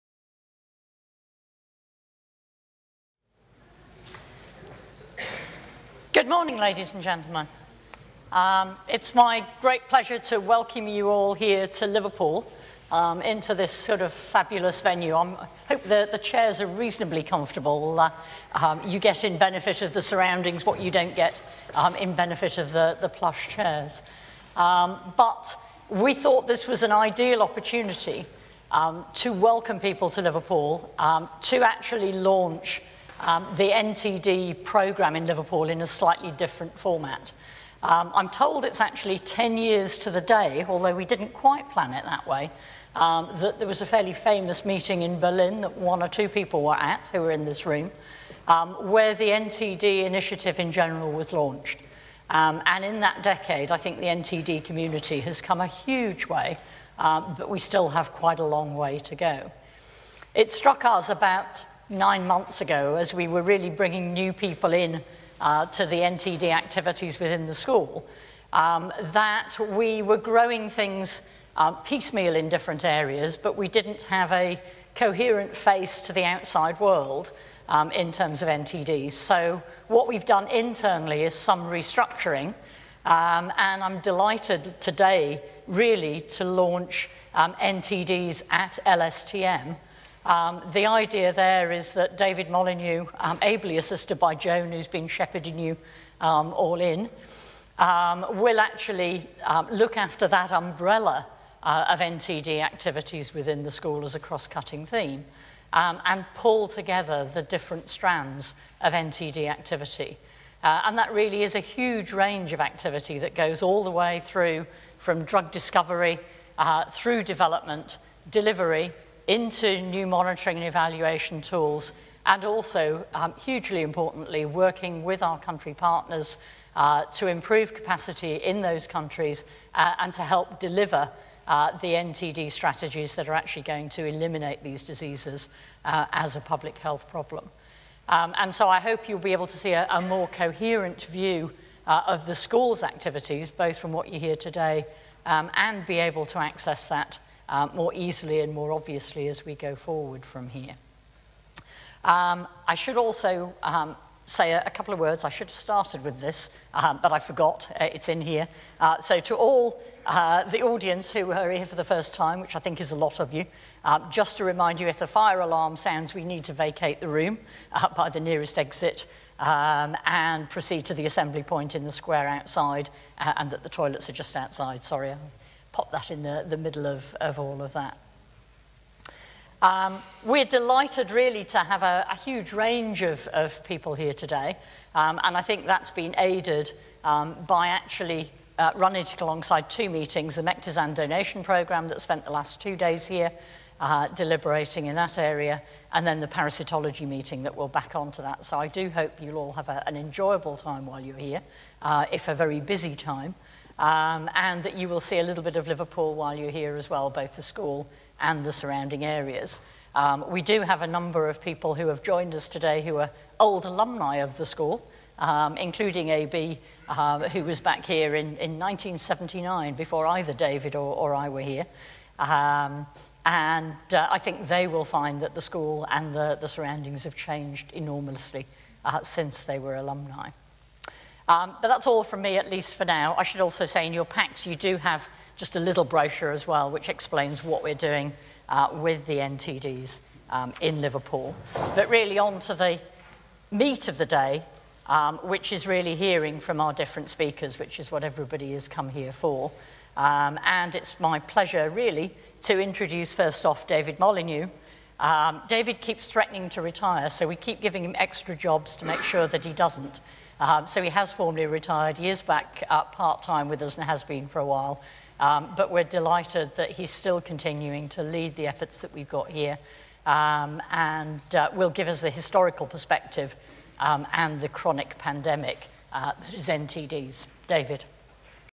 NTD a cross-cutting theme at LSTM. Professor Janet Hemingway
Addressing the chronic pandemic of neglected tropical diseases the first decade & onwards
Wednesday, 15th April 2015 - Leggate Theatre, Victoria Gallery & Museum
Meeting organised by LSTM - Neglected Tropical Diseases